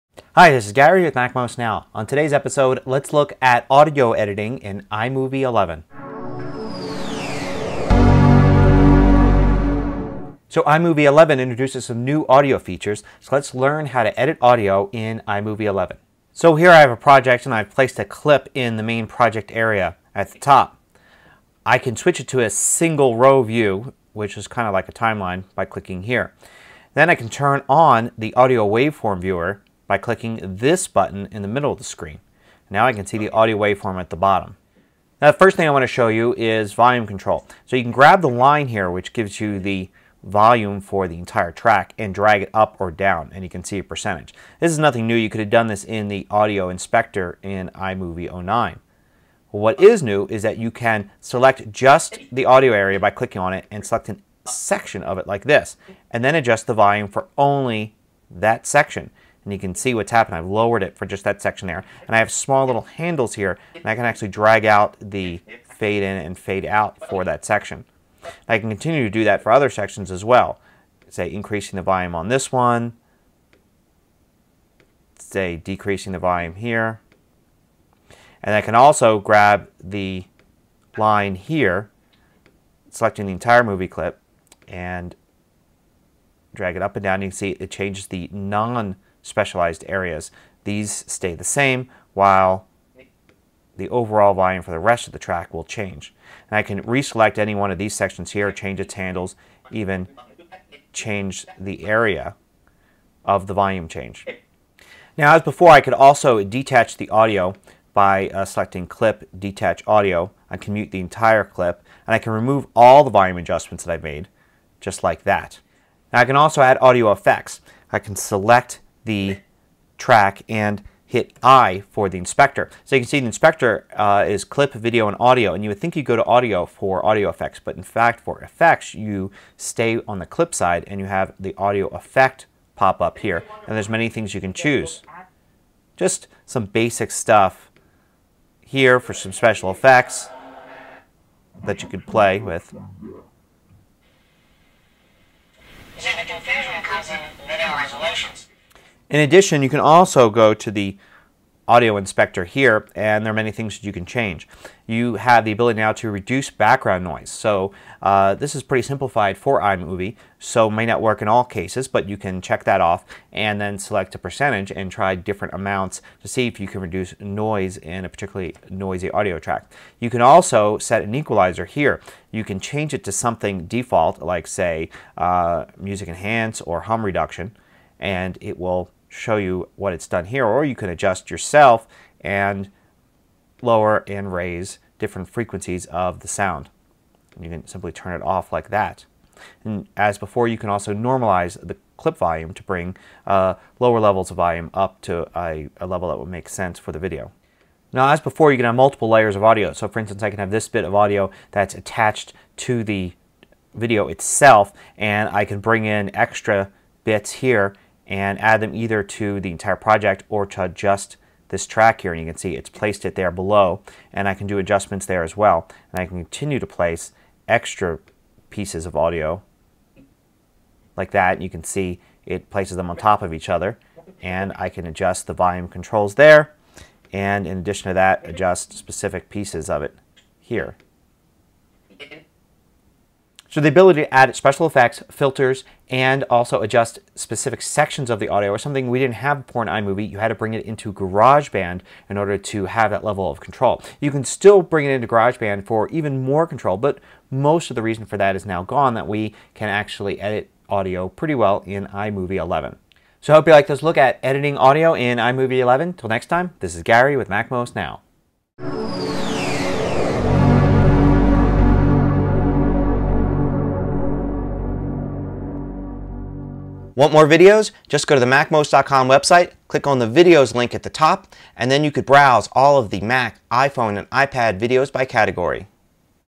Editing Audio in iMovie 11 (MacMost Now 468)
iMovie 11 allows you to set the volume for specific portions of the audio track. You can adjust the sections of volume control and set fade in and out times for each section. You can also apply audio effects, filter sound using an equalizer and layer multiple tracks of audio.